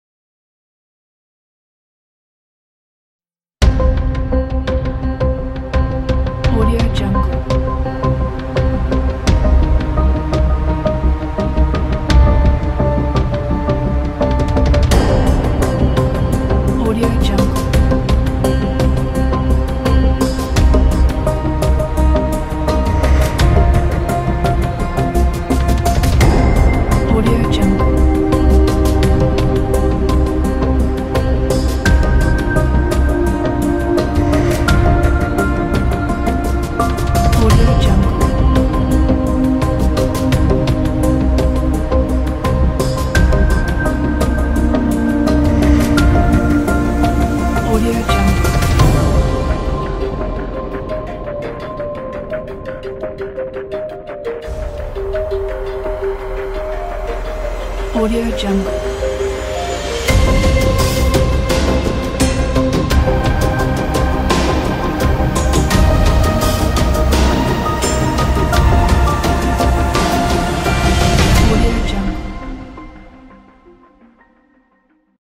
Tech Slides (After Effects template)
17 photo placeholders, 17 text placeholders
Project has good structure and it’s very easy to use
All main colors can be adjusted